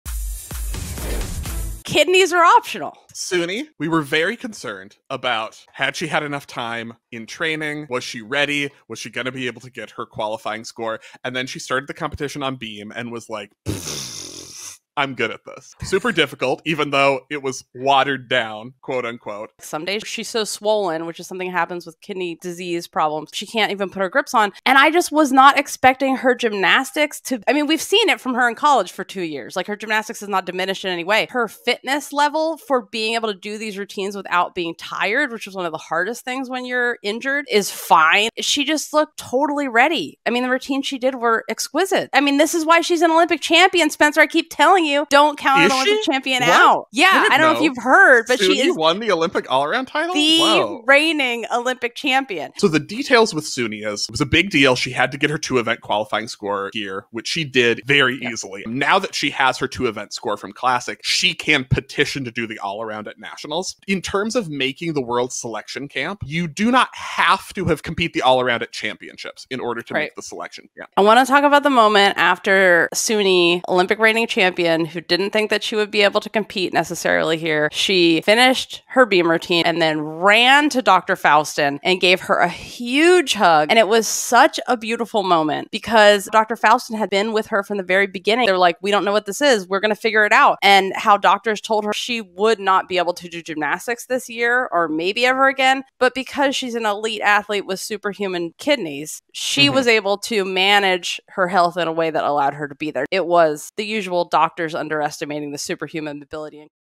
Suni's Kidneys Are Optional
Sunisa Lee’s remarkable journey battling kidney challenges with grace at the U.S. Classic.